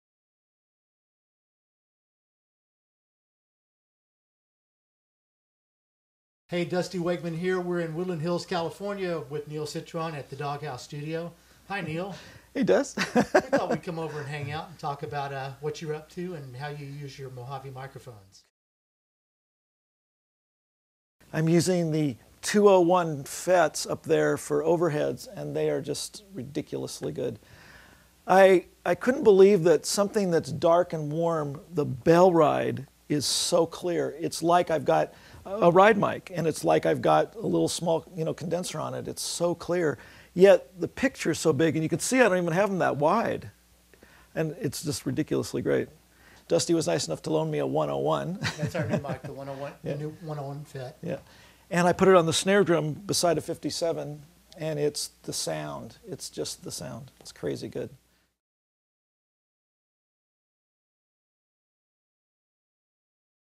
Mojave presents Neil Citron - Teaser Trailer
Neil Citron shares his thought on Mojave Audio microphones and recording.